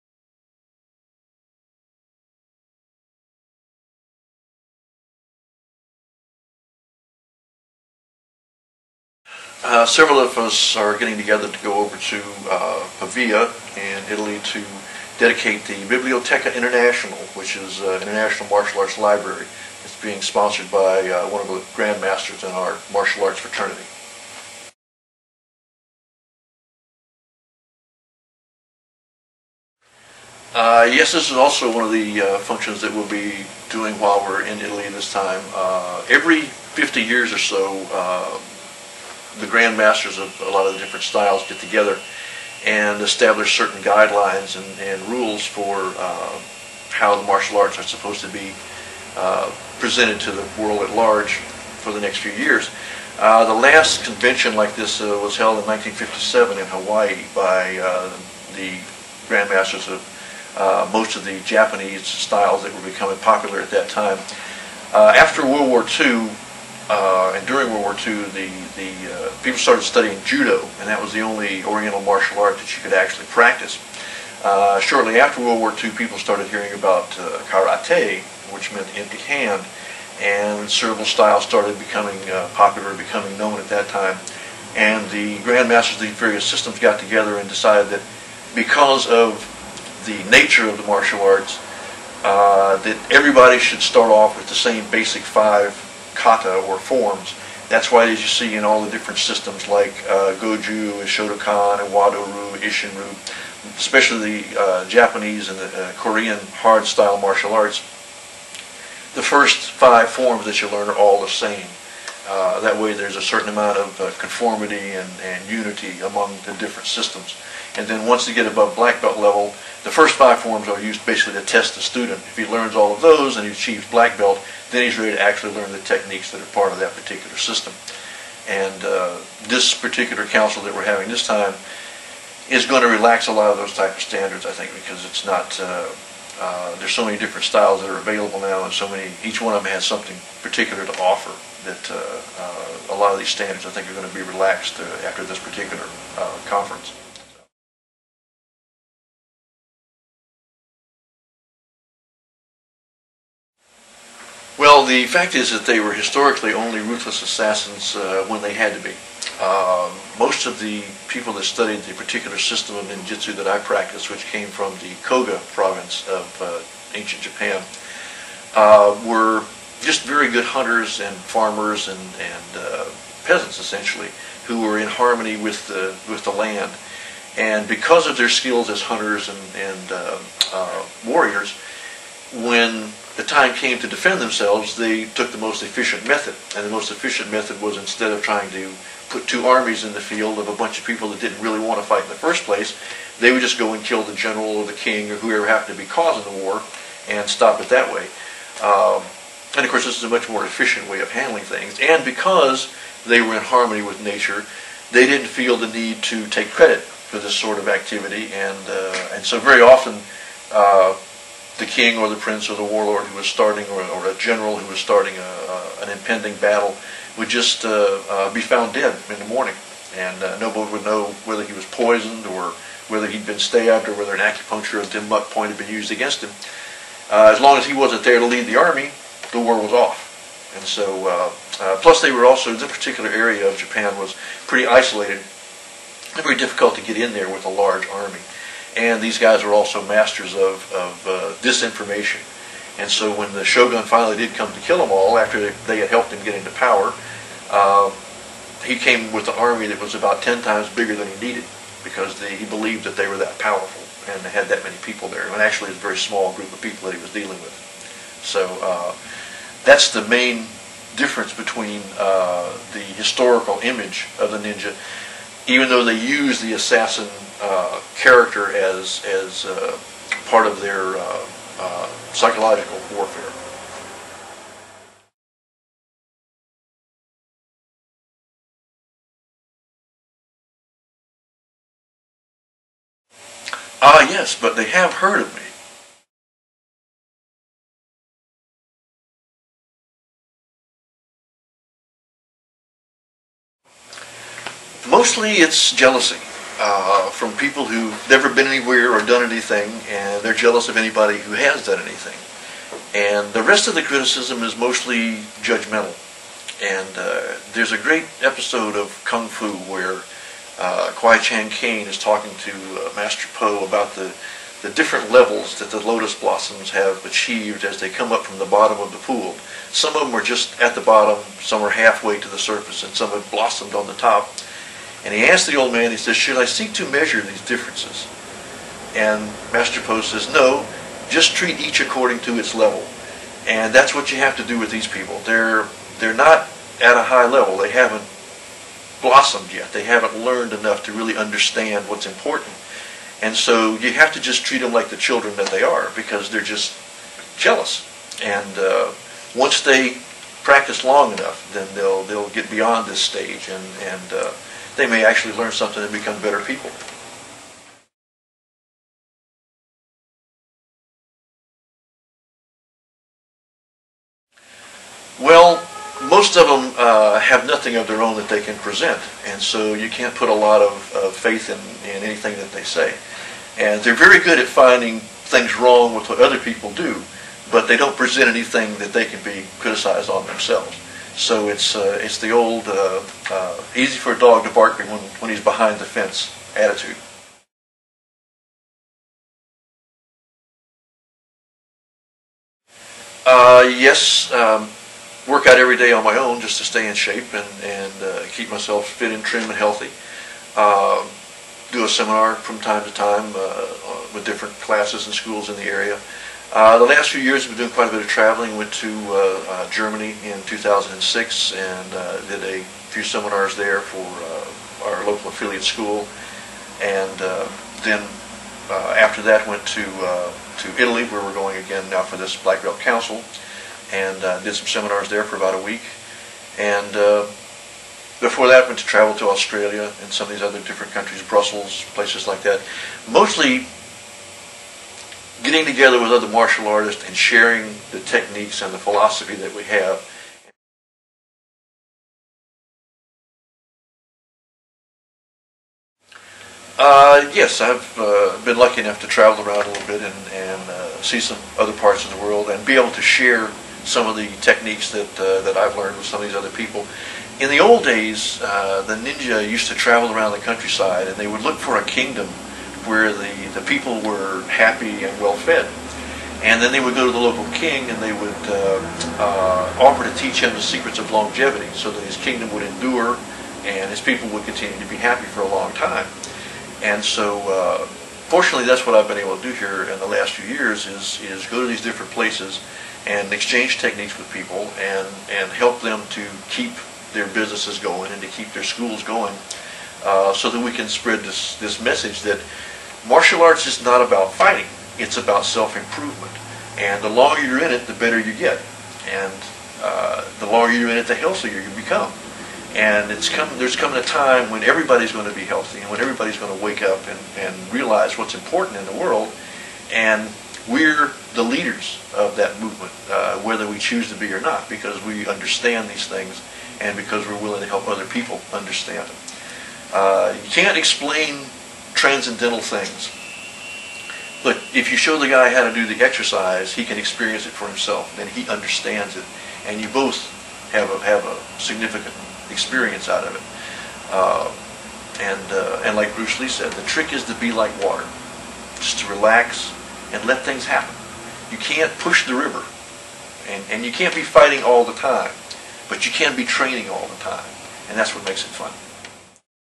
Ashida Kim Interview March 01 2007 Raw Footage
Independent report interviews with grandmaster Ashida Kim about his tour of Euorpe this year and his "Critics" on the internet. Visit Grandmaster Ashida Kim online at AshidaKim.com  or his book store at Dojopress.com